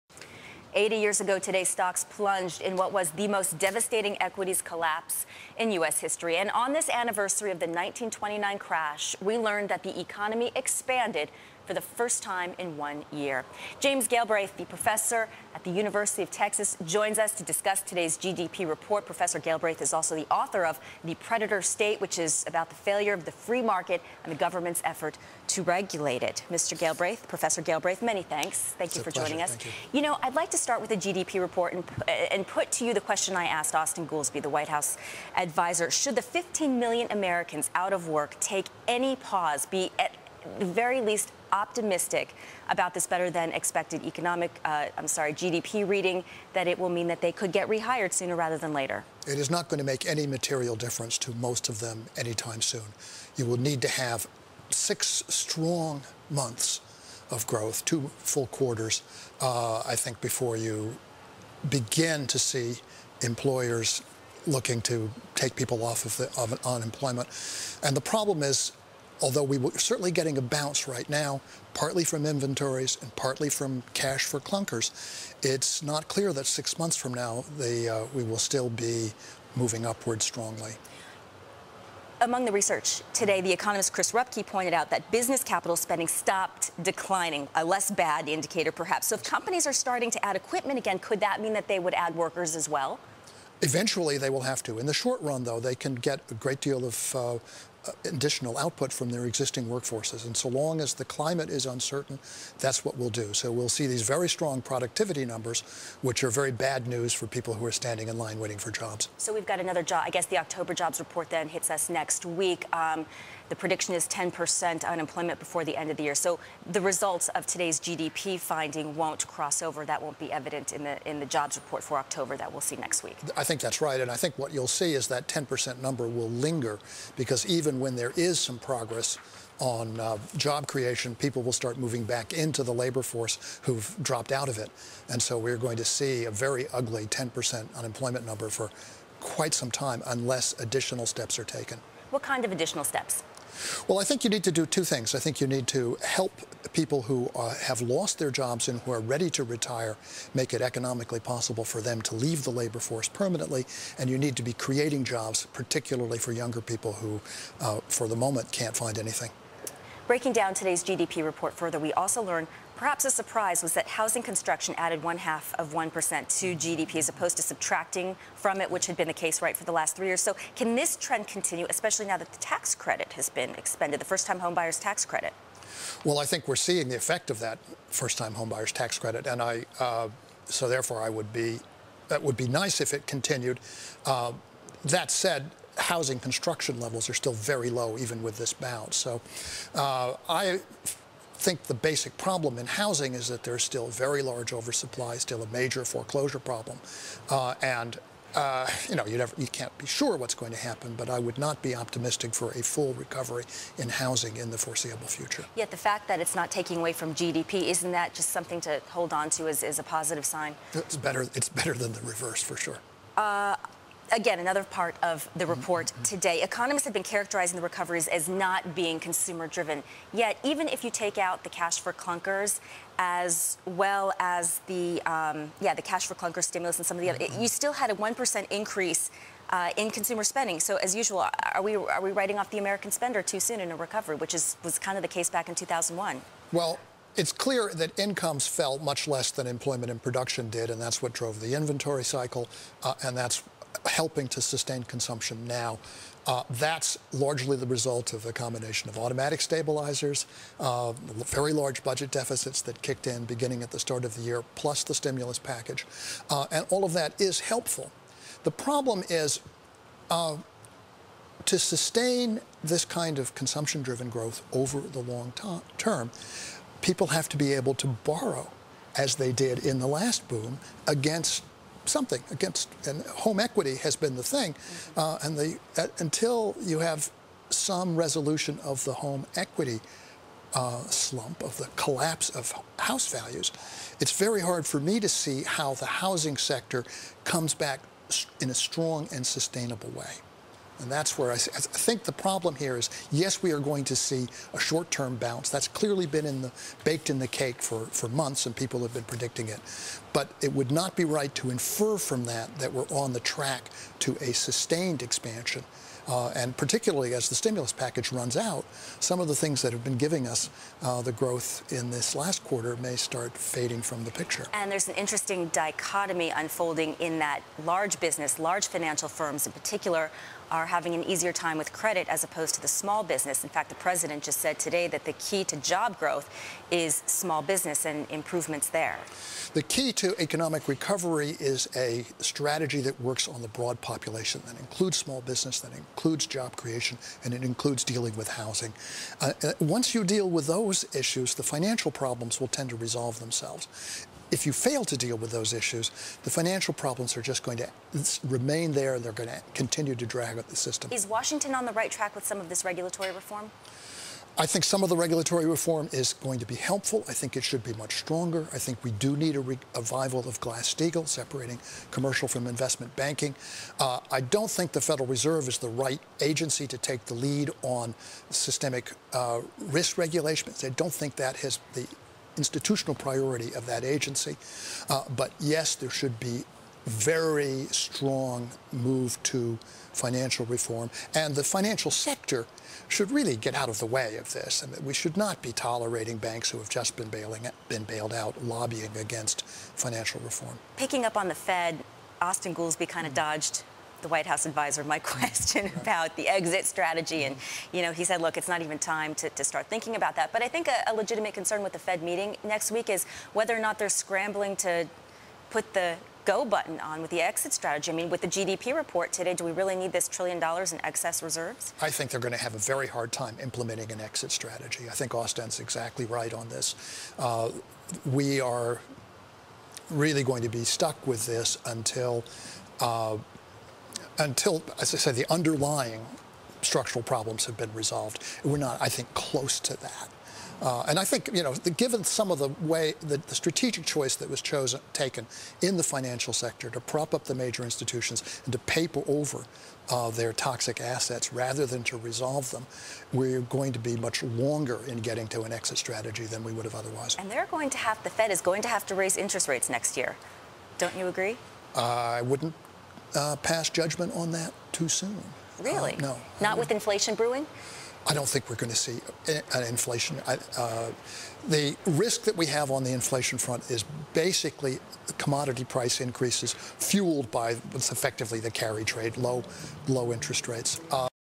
Galbraith Expects U.S. Unemployment to `Linger' at 10%: Video
Oct. 29 (Bloomberg) -- James Galbraith, an economics professor at the University of Texas in Austin, talks with Bloomberg's Lori Rothman about the U.S. economy and labor market. (This report is an excerpt. Source: Bloomberg)